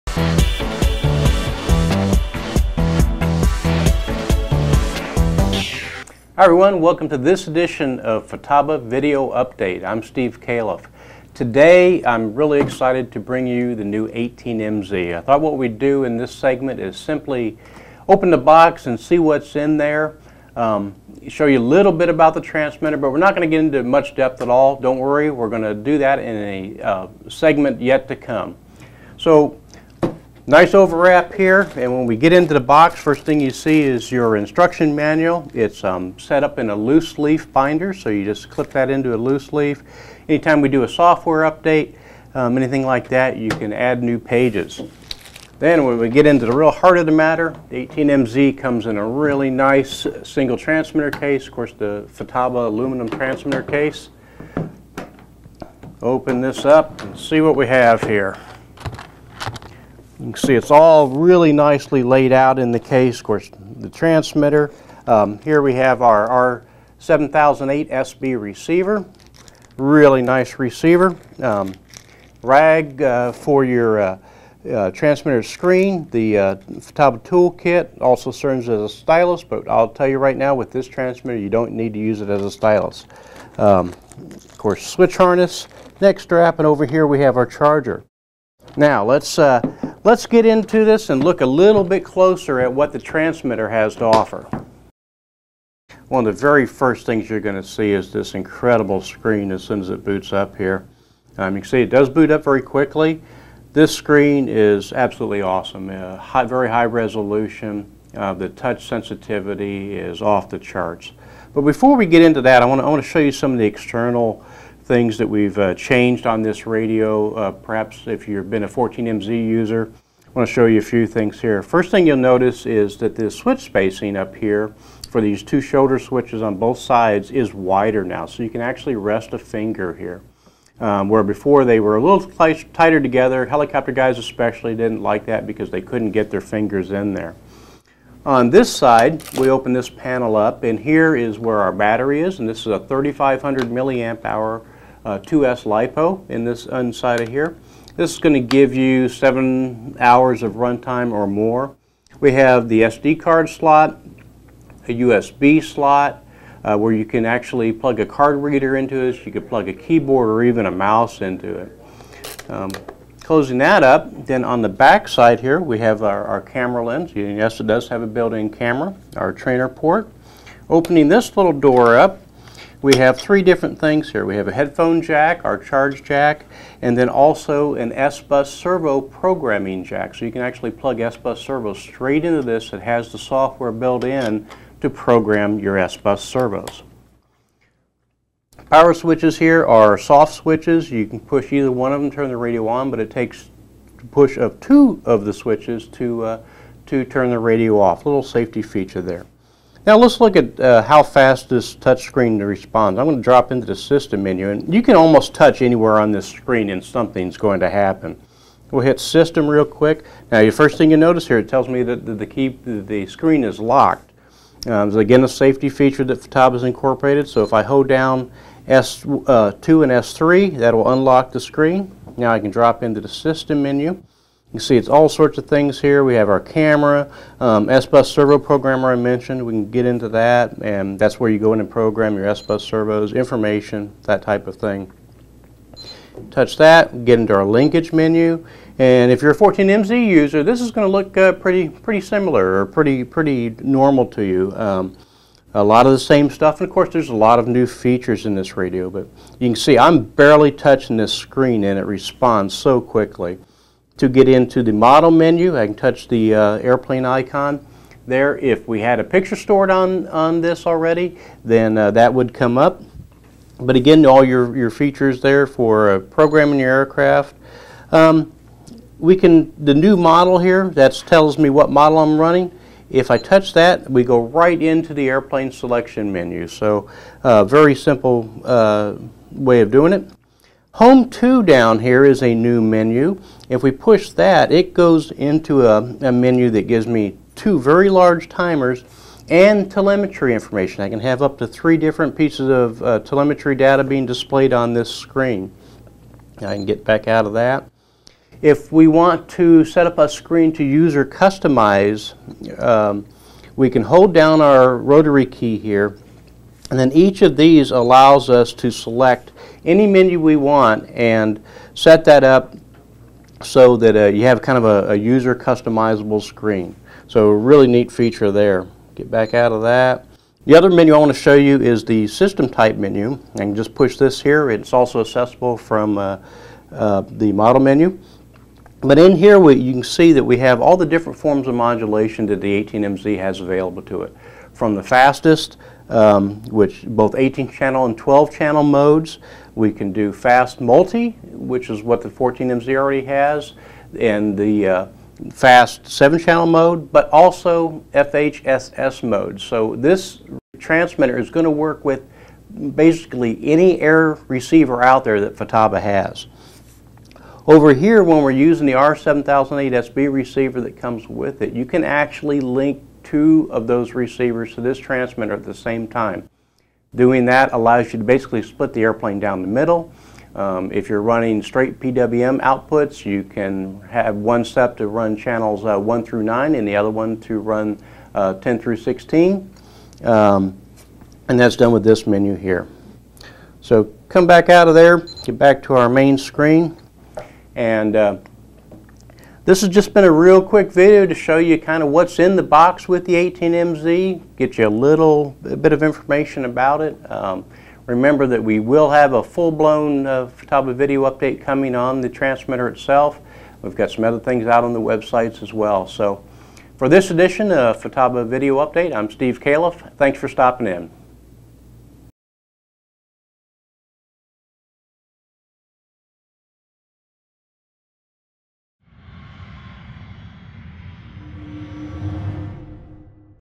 Futaba® Video Update - 18MZ 18-Channel 2.4GHz FASST Computer Radio
It's going to change the way 2.4GHz systems are designed — and take your radio control experience to new heights! Futaba's first true 18-channel radio, the 18MZ is the most advanced 2.4GHz system in the world, with FASSTest technology that makes it fully backwards-compatible with all FASST, FHSS and S-FHSS systems. It's also bi-directional, which means it can support a wide range of telemetry functions including RPM, temperature, voltage, altitude, GPS and fuel levels. The included R7008SB high-voltage receiver lets you use as many channels as you want. Other key features include a 6" touch screen, built-in camera, two USB ports, a 7.4V LiPo battery, SD card slot, serial interface, Easy Link ID™ and much more.